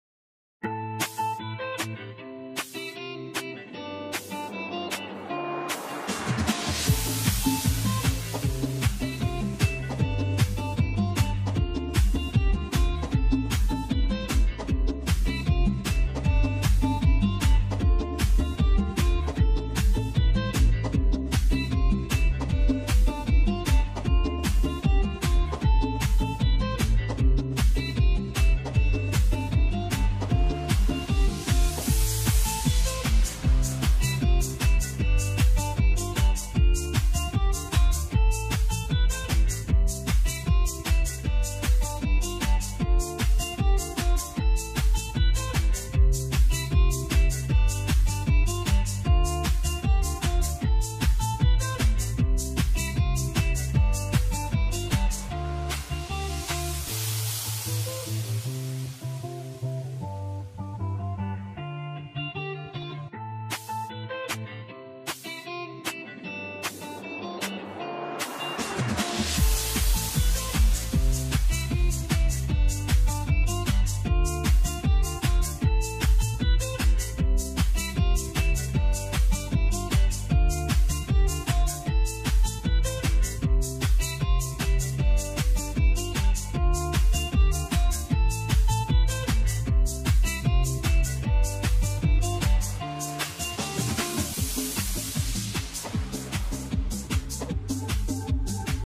2017 Semester 1, Weeks 4 & 5
It's been go, go, go here at IHD! The wet season may be hanging around, but Residents have been competing in soccer comps and made their way to the Tiwi Islands Footy Grand Final & Art Show. Take a look...